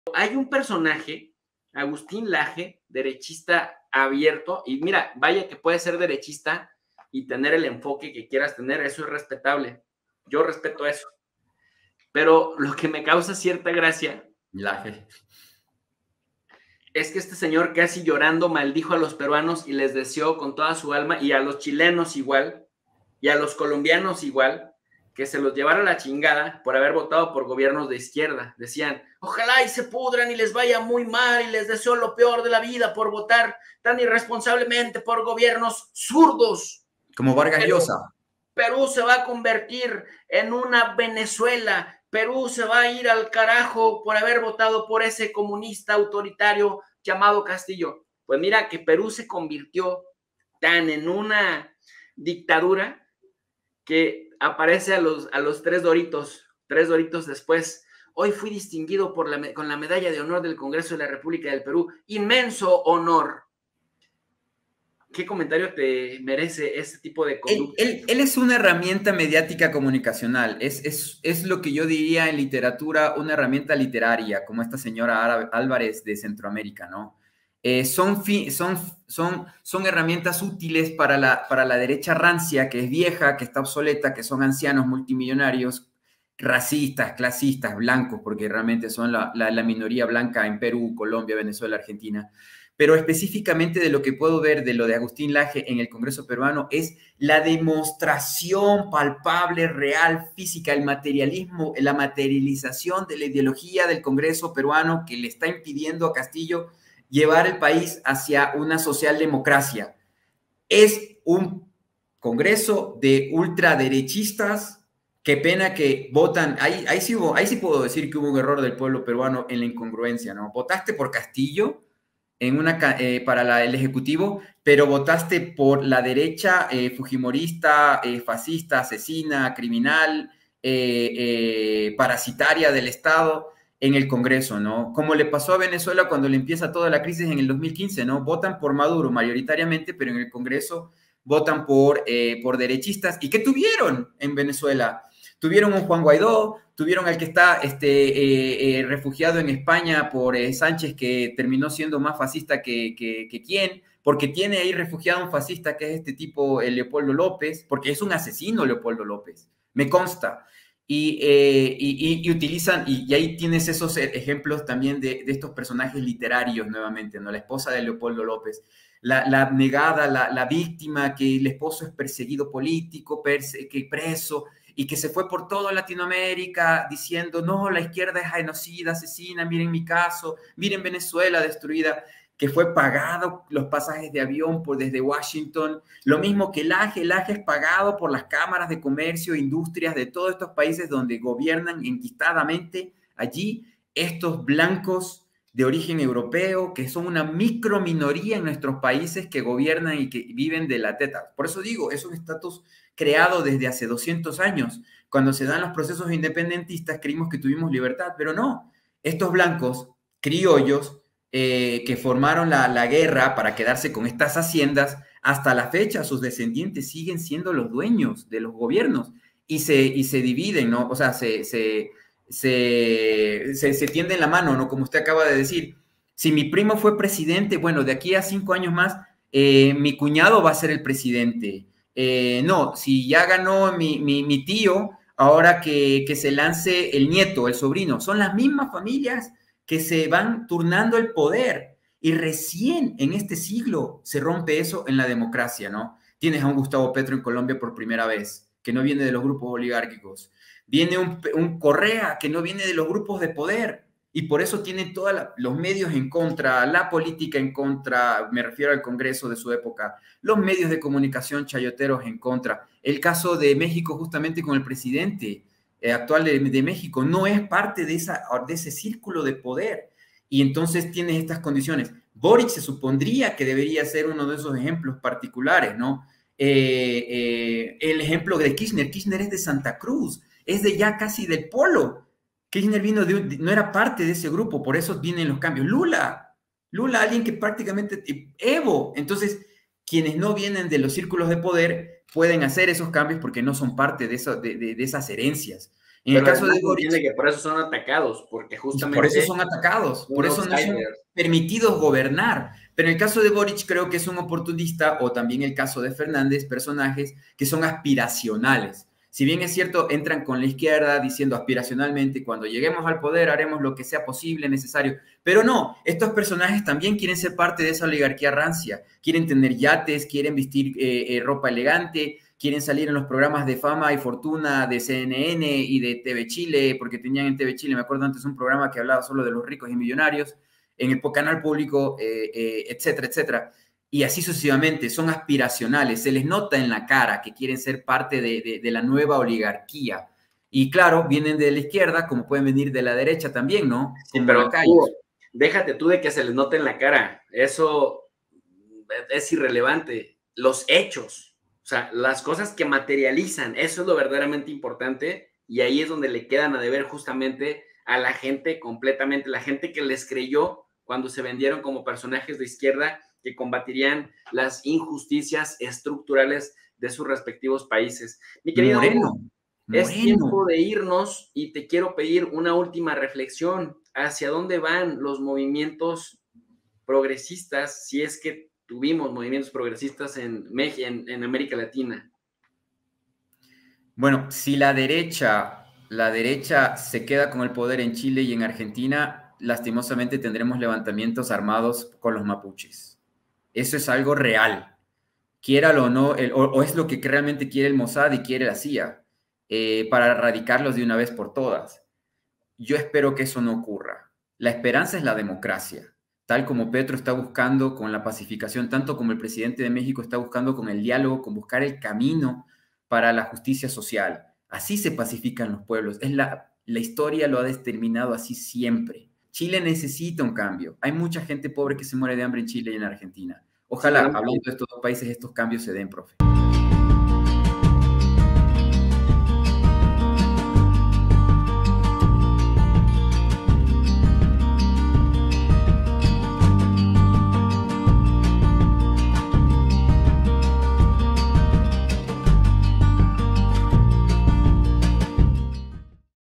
¿POR QUÉ SON ATACADOS LOS GOBIERNOS DE IZQUIERDA EN AMÉRICA LATINA?
En este video nuestro amigo Hugo Godoy nos comparte su reflexión sobre los gobiernos de izquierda en América Latina y los ataques que han sufrido por parte de la derecha.